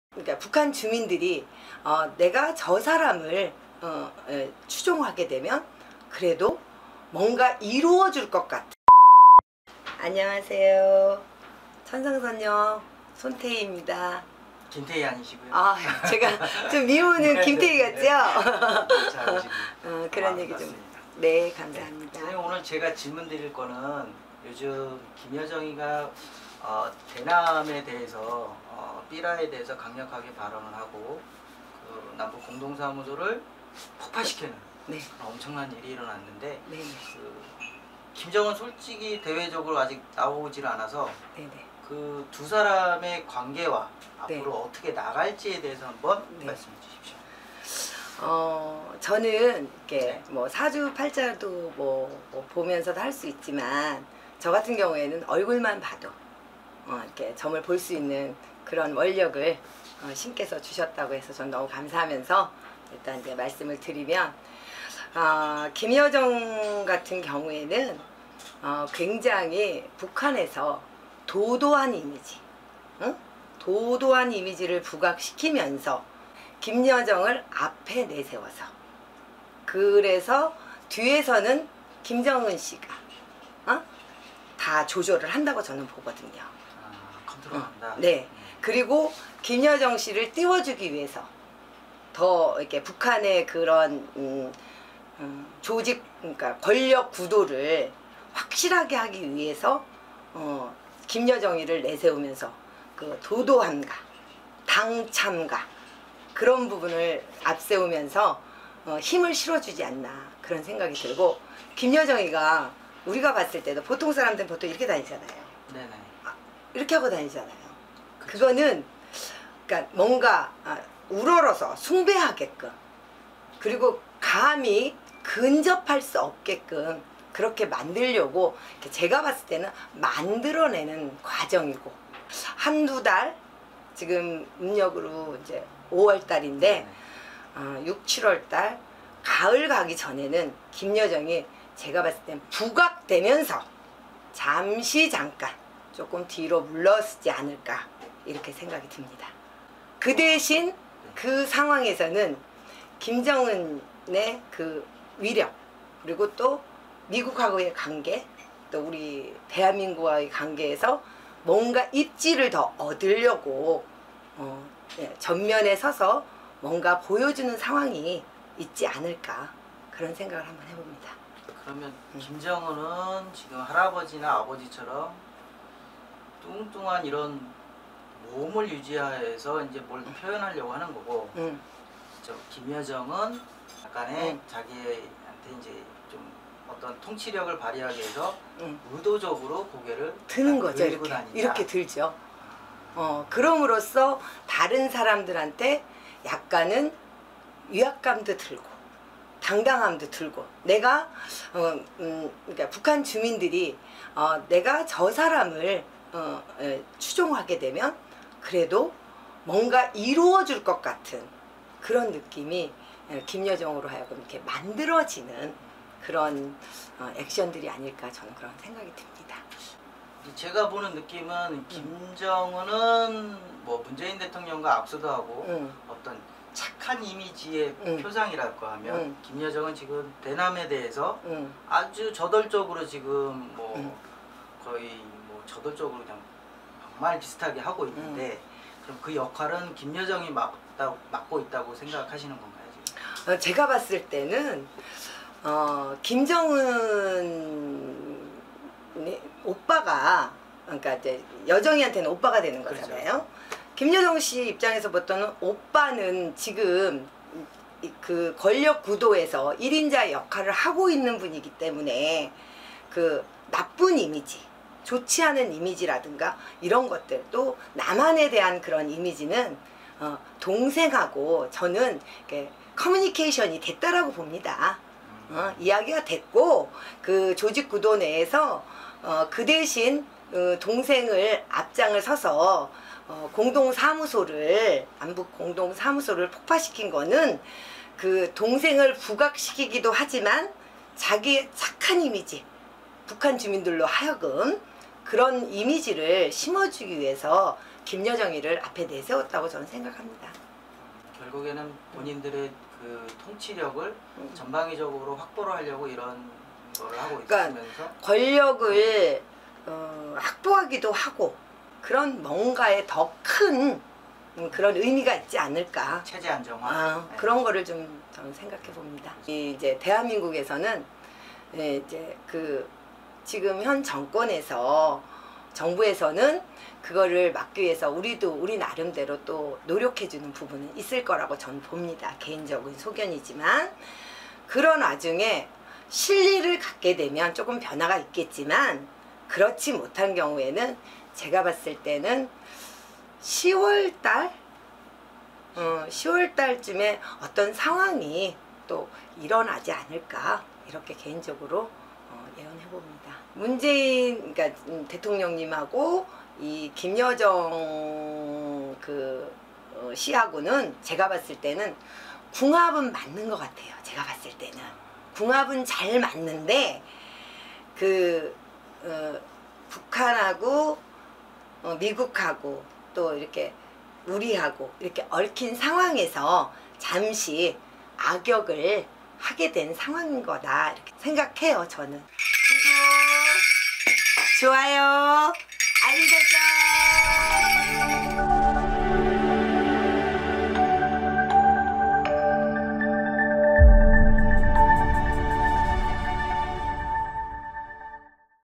노원구점집 천상선녀가 본 김여정 역할과 대북관계
최근 삐라 , 남북공동연락사무소 폭팔 대남 강경자세를 유지하고 있는 김여정
천상선녀 손태희 선생님이 바라 본 김여정의 속내와 향후 대북관계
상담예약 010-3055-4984